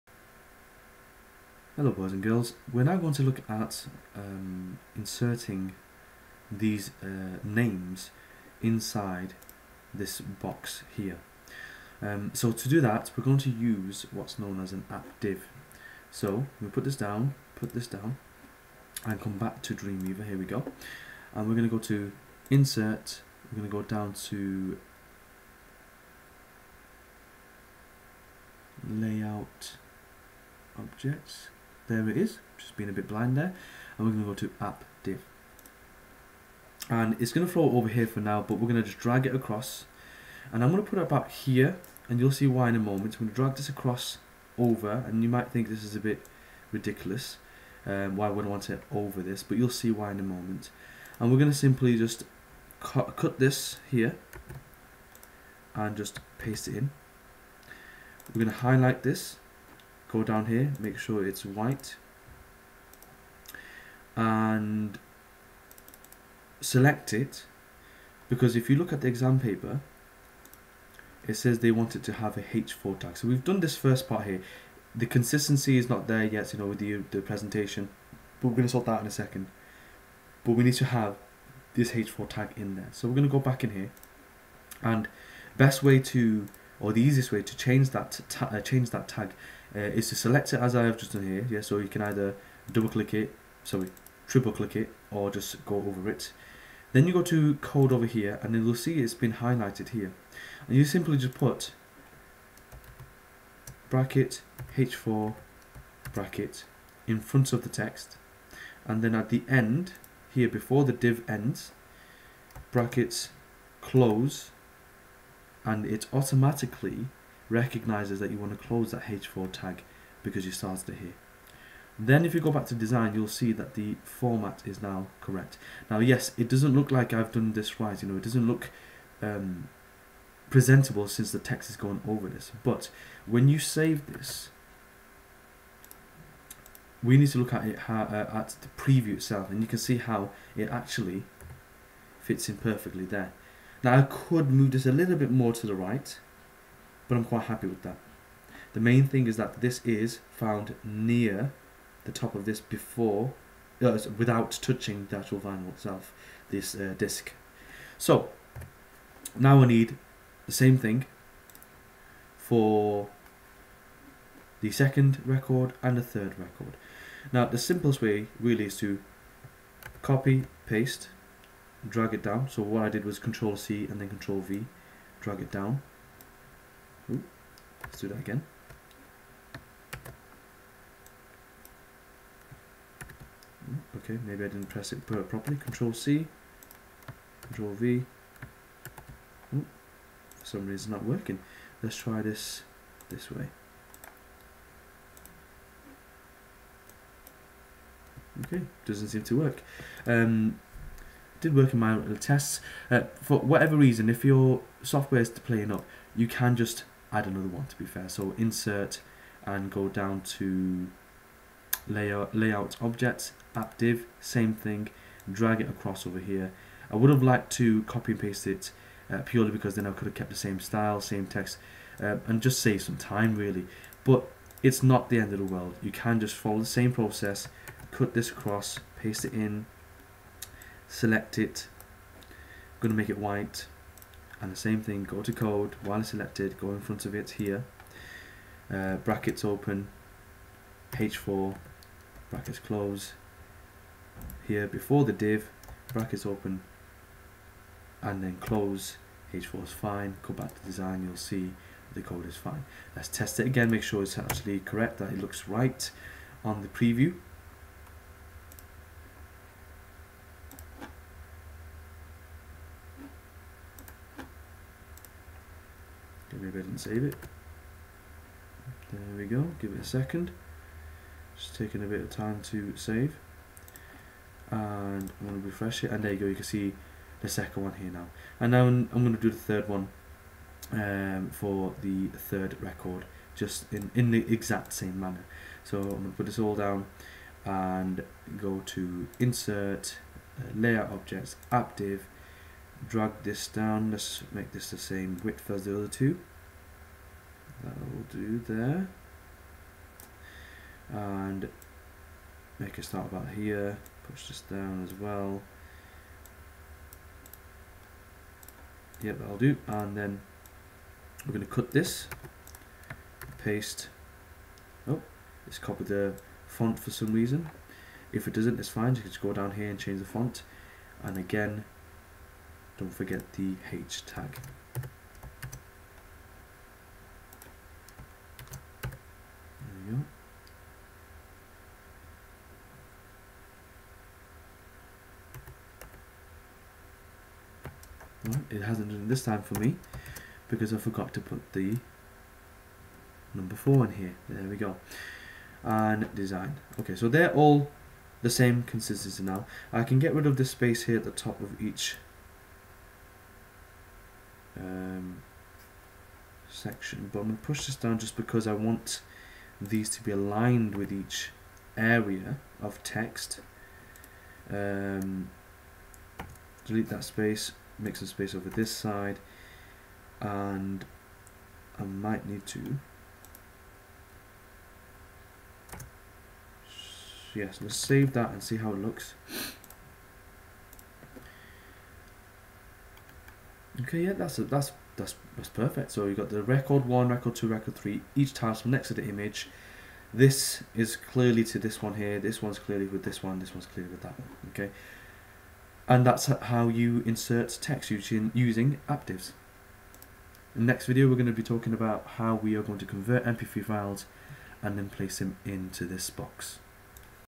13 Using Ap Divs to insert text
CiDA Unit 1 Exam: Developing Web Products. (9 January – 13 January 2017)

A step-by-step guide.

This series will go through the 2017 exam paper and aims to break down the key tasks into smaller easy to digest chunks. If you would like to follow along from home then you can find the ASSETS

The software applications used in this series include: Adobe Dreamweaver CS5.5, Adobe Photoshop CS5.5, Adobe Audition CS5.5 and Adobe Fireworks CS5.5.